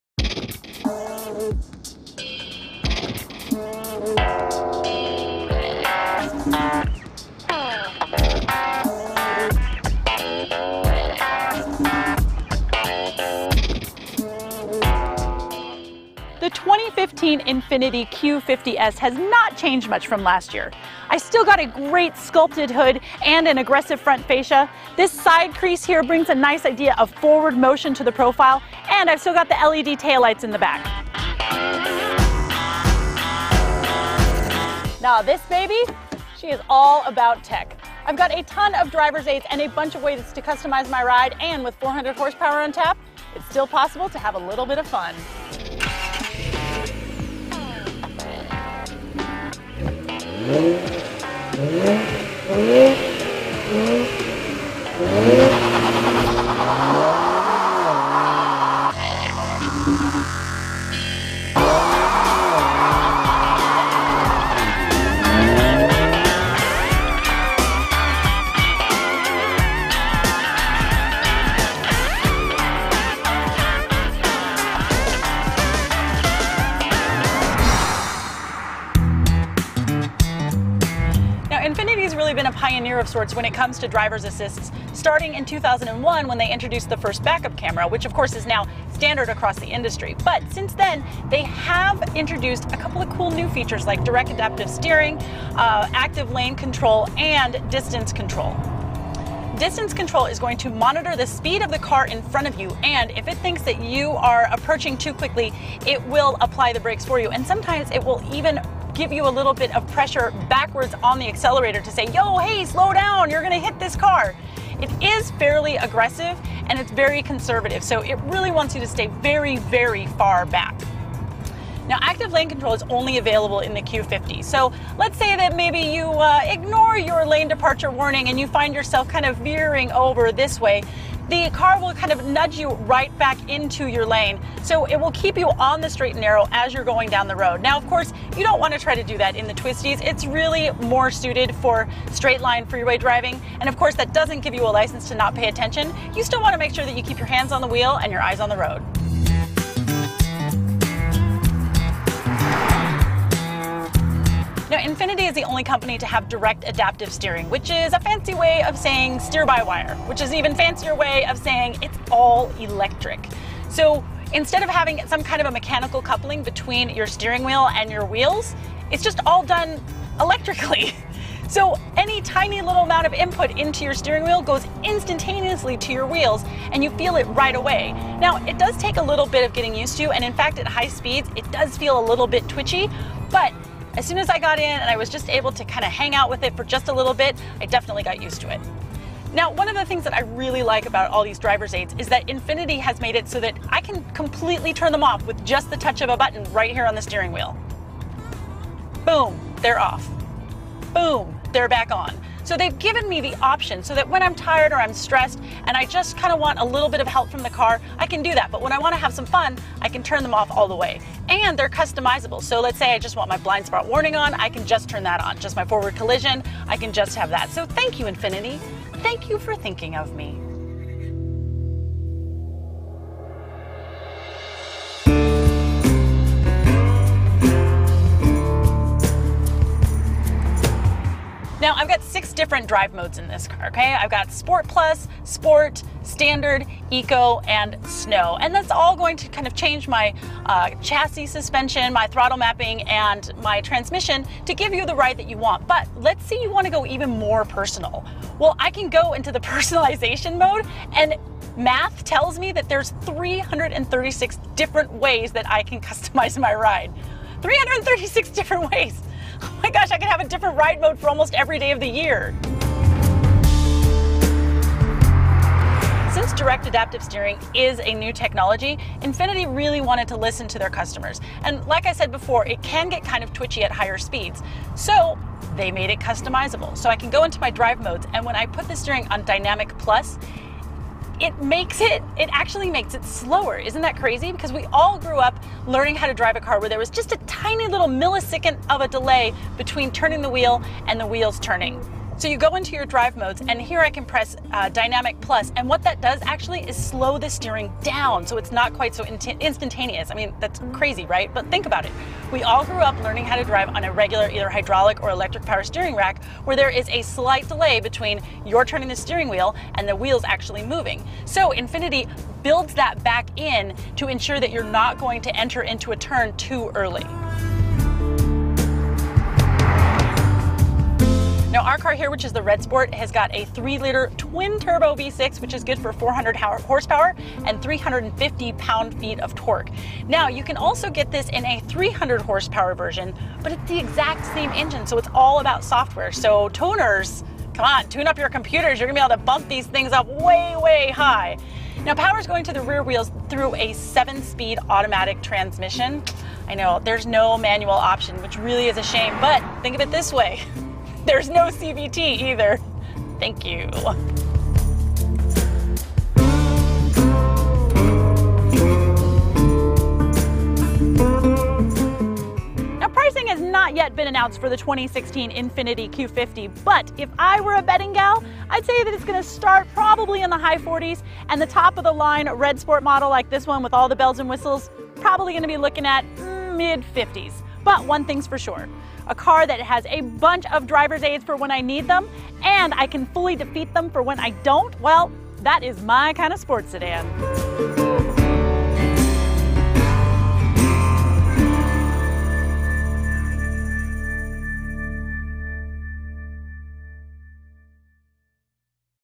The Infiniti Q50s is like having more than 300 cars in one
With a multitude of unique driving modes, the 2016 Infiniti Q50s can be customized 336 ways for a ride that is all your own.